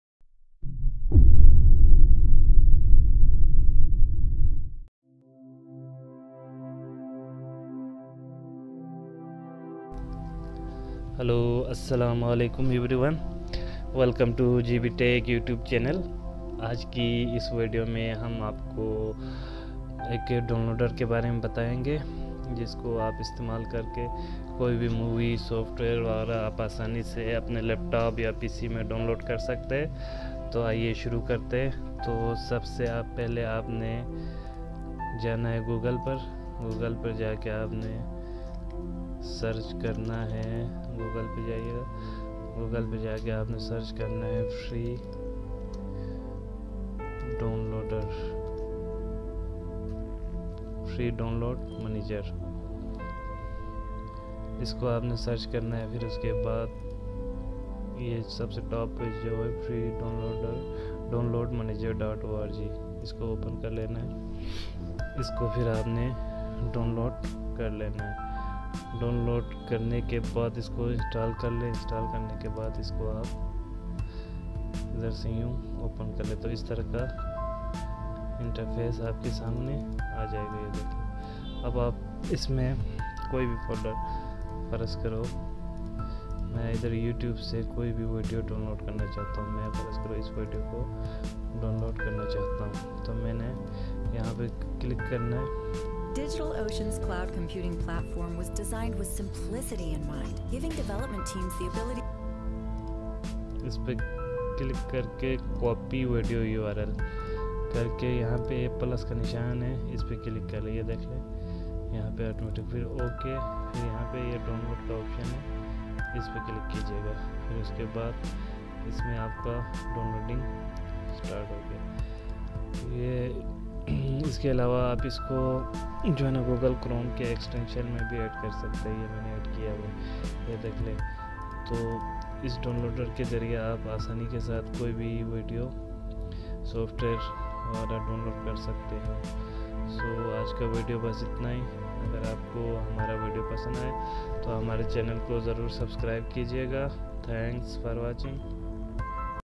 Download Movie ,Software Without idm || Best Software To Download Anything Fastly
hi everyone in this video i will show you how to download movies .software etc without idm
best downloader to download any thing